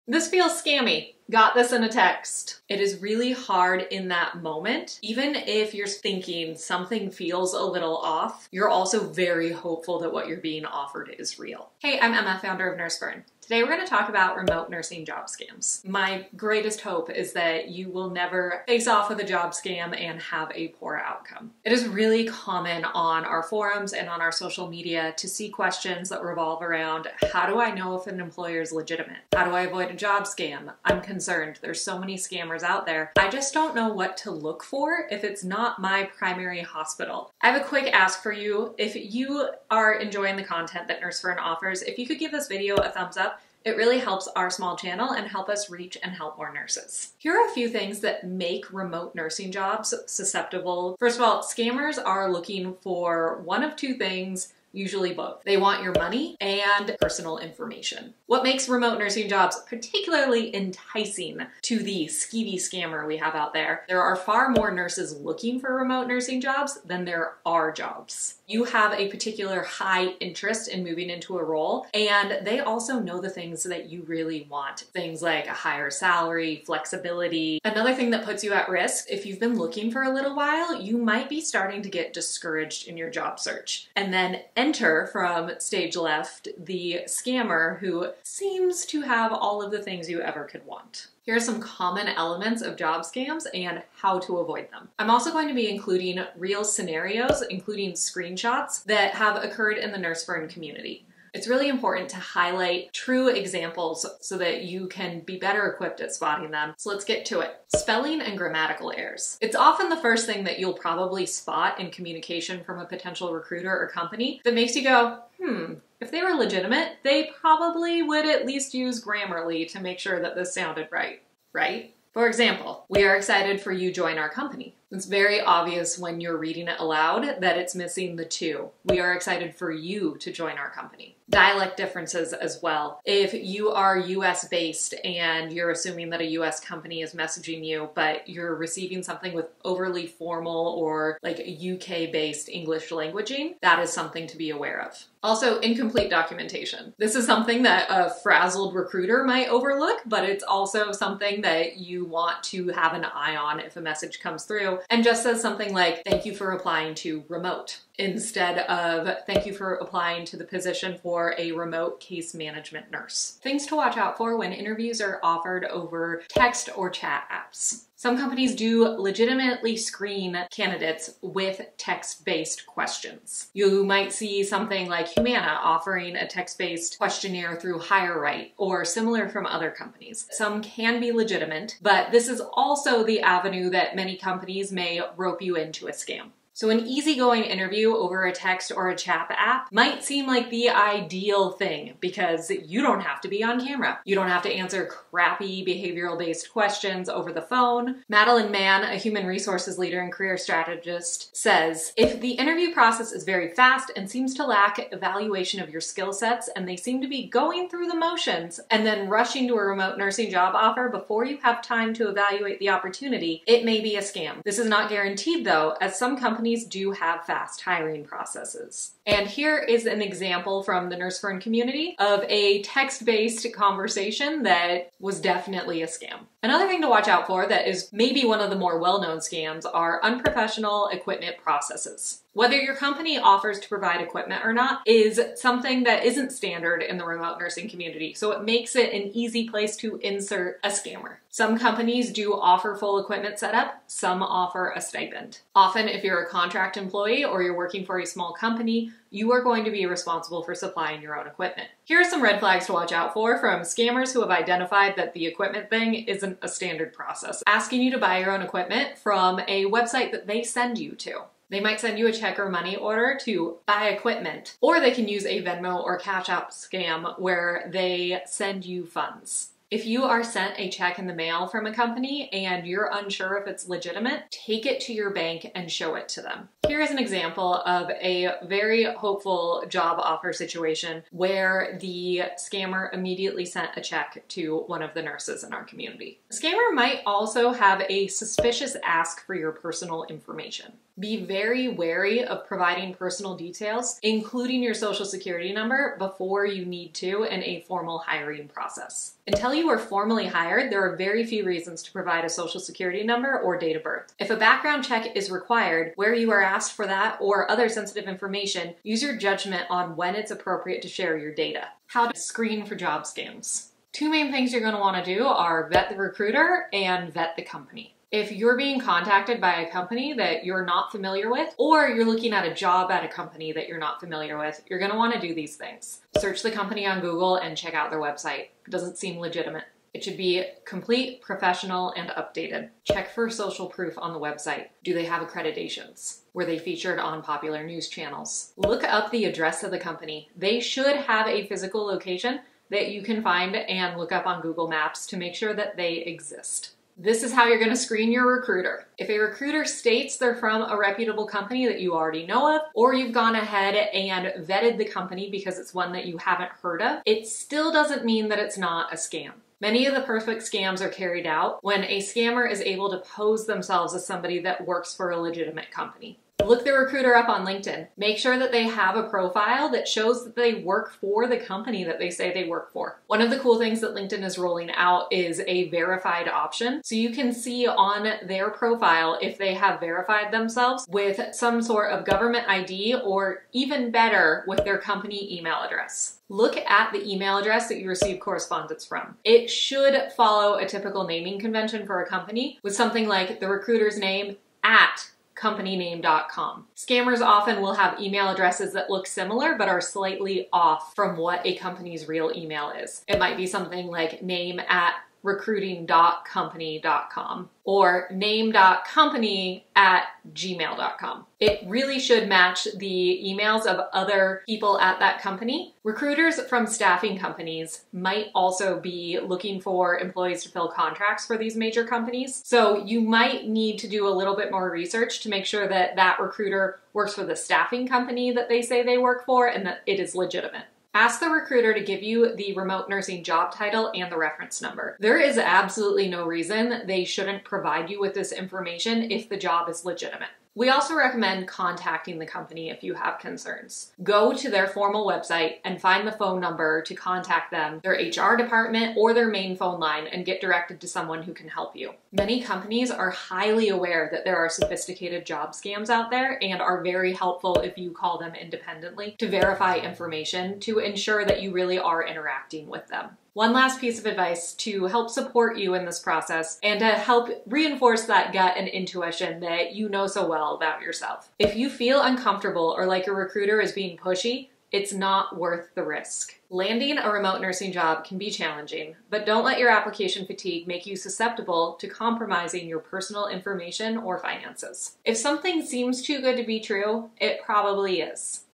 Remote Nurse Job Scams: Avoid These Red Flags
How do you avoid scams and find legitimate companies?

Remote nursing job scams exist, but they shouldn’t keep you from pursuing a non-bedside or remote nursing job. Knowing the signs of a scam and how to vet companies and recruiters will give you the confidence to navigate the job search market.

Highlighted Resources
👉Read the full article Remote Nurse Job Scams: Avoid These Red Flags

🫶Take the free quiz to see what remote nursing job might be right for you!